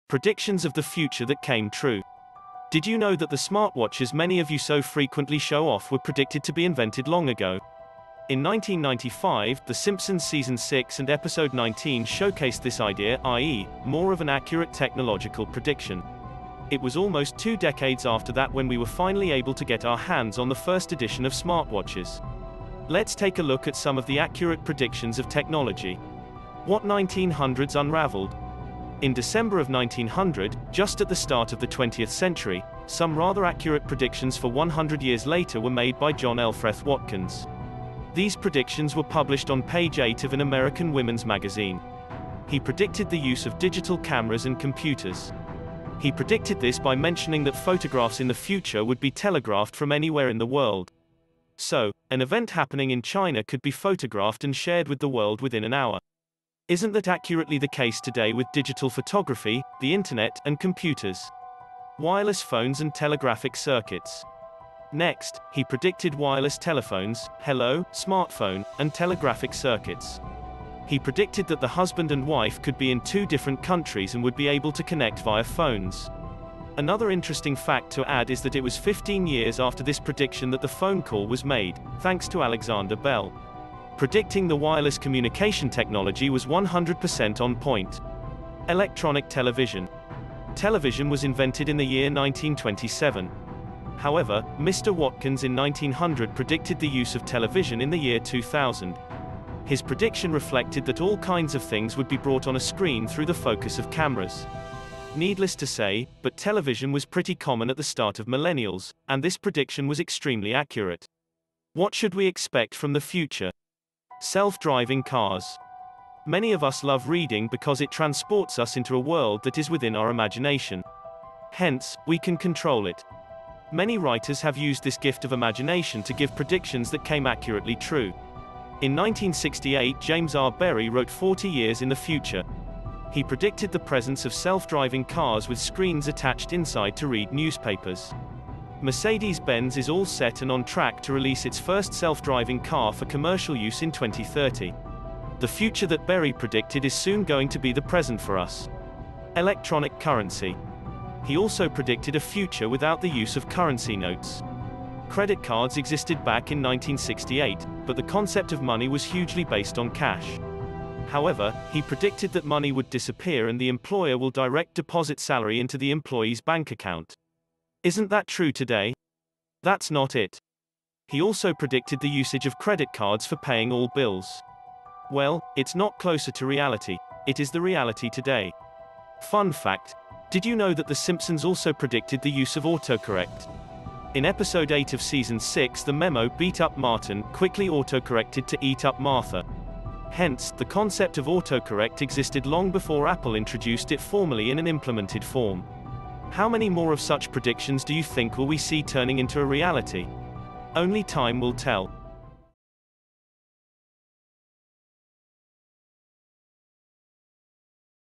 Predictions of The Future that Came True | Shifu Digital
Did you know that the smartwatches many of you so frequently show off were predicted to be invented long ago? In 1995, The Simpsons season 6 and episode 19 showcased this idea, i.e., more of an accurate technological prediction. It was almost two decades after that when we were finally able to get our hands on the first edition of smartwatches.
Let's take a look at some of the accurate predictions of technology.

What 1900s unraveled?
In December of 1900, just at the start of the 20 th century, some rather accurate predictions for 100 years later were made by John Elfreth Watkins. These predictions were published on page eight of an American Women&#39;s Magazine. He predicted the use of digital cameras and computers. He predicted this by mentioning that photographs in the future would be telegraphed from anywhere in the world. So, an event happening in China could be photographed and shared with the world within an hour. Isn&#39;t that accurately the case today with digital photography, the internet, and computers?

Wireless phones and telegraphic circuits:
Next, he predicted wireless telephones (Hello, Smartphone!!) and telegraphic circuits. He predicted that the husband and wife could be in two different countries and would be able to connect via phone. Another interesting fact to add is that it was 15 years after this prediction that the phone call was made, thanks to Alexander Bell. Predicting the wireless communication technology was 100% on point.

Electronic television:
Television was invented in the year 1927. However, Mr. Watkins 1900 predicted the use of television
in the year 2000. His production reflected that all kinds of things would be brought on a screen through the focus of cameras.
Needless to say, but television was pretty common at the start of millennials, and this prediction was extremely accurate.

What should we expect from the future?

Self-driving cars:
Many of us love reading because it transports us into a world that is within our imagination. Hence, we can control it. Many writers have used this gift of imagination to give predictions that came accurately true. In 1968 James R. Berry wrote 40 years in the future. He predicted the presence of self-driving cars with screens attached inside to read newspapers. Mercedes Benz is all set and on track to release its first self-driving car for commercial use in 2030. The future that Berry predicted is soon going to be the present for us.

Electronic currency:
He also predicted a future without the use of currency notes. Credit cards existed back in 1968, but the concept of money was hugely based on cash. However, he predicted that money would disappear and the employer will direct deposit salary into the employee's bank account. Isn't that true today? That's not it. He also predicted the usage of credit cards for paying all bills. Well, it's not closer to reality; it is the reality today.

Fun fact: Did you know that The Simpsons also predicted the use of autocorrect?
In episode 8 of season 6 the memo &quot; beat up Martin&quot; quickly autocorrected to & quot; eat up Martha&quot;. Hence, the concept of autocorrect existed long before Apple introduced it formally in an implemented form. How many more of such predictions do you think will we see turning into a reality? Only time will tell.

Write in the comments what the video looks like and what region of the world you are watching from.